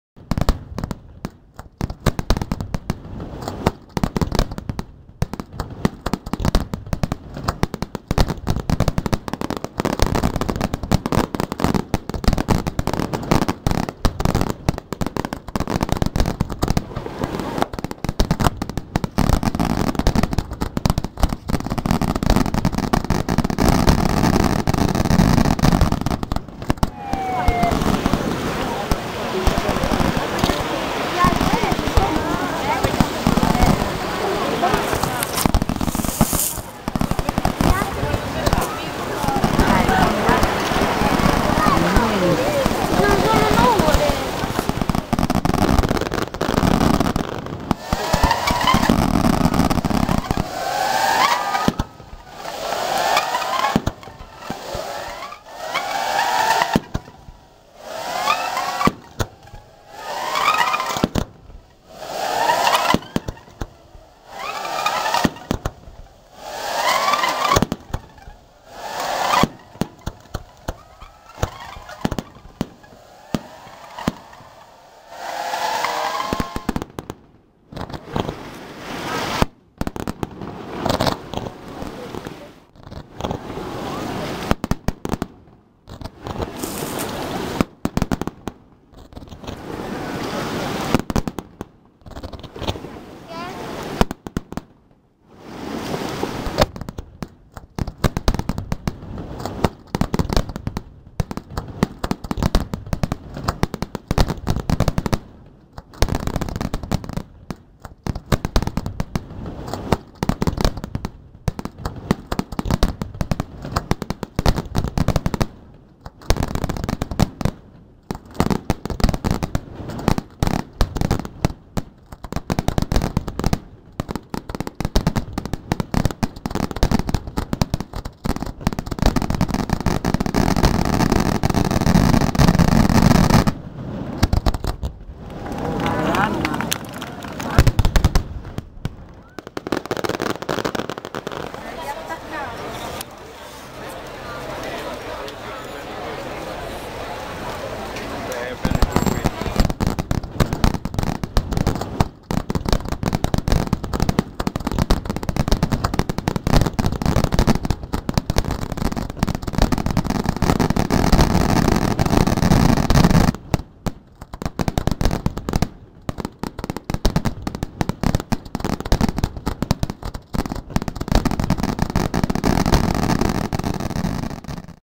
La riviera del Conero illuminata a ferragosto.
La riviera del Conero spettacoli pirotecnici.
La vigilia di ferragosto processione e fuochi
artificiali a Numana, la sera di ferragosto
il grande appuntamento a Porto Recanati.